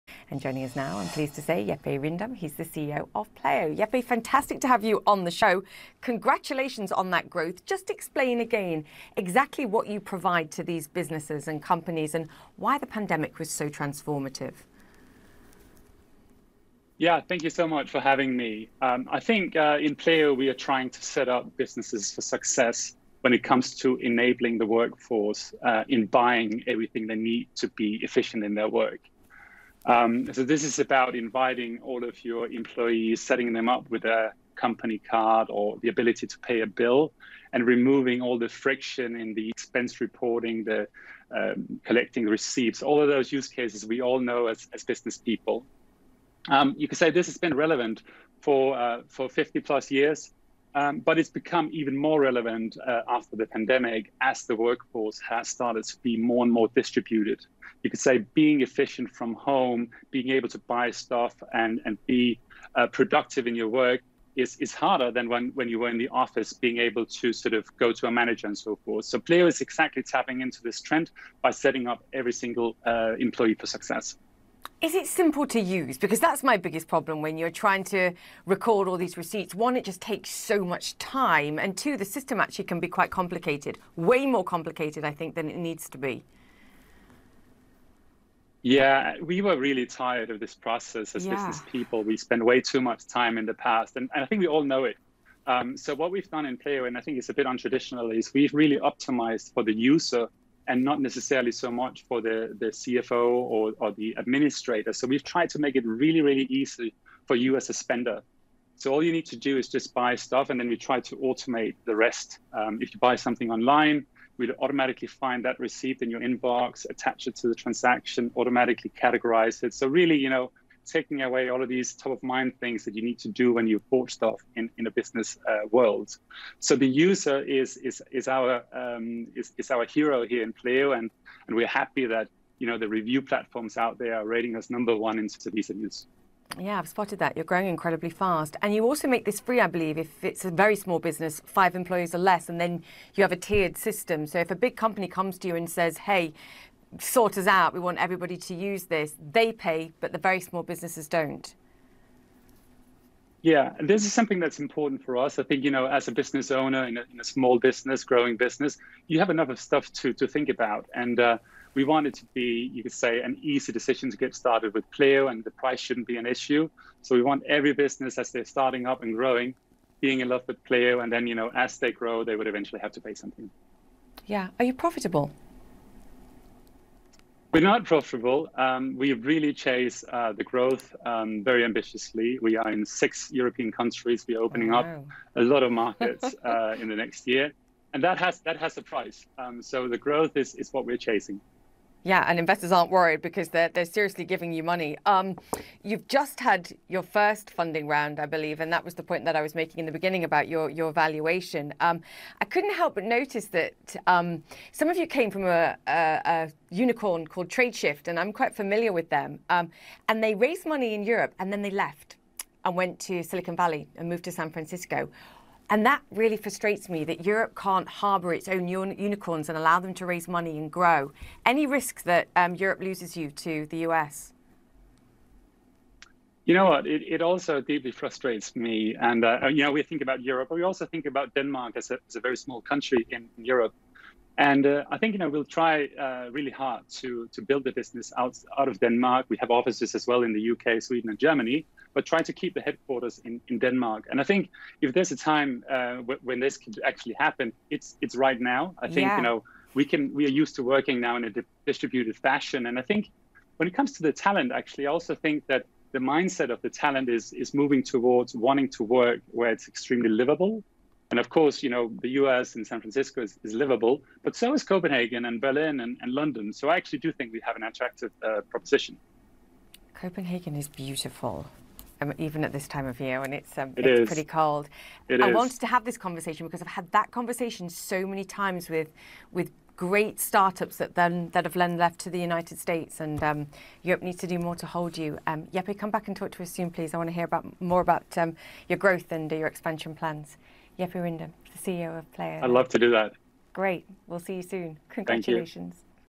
Jeppe Rindom interview CNN
Julia Chatterley interviews Jeppe Rindom from Pleo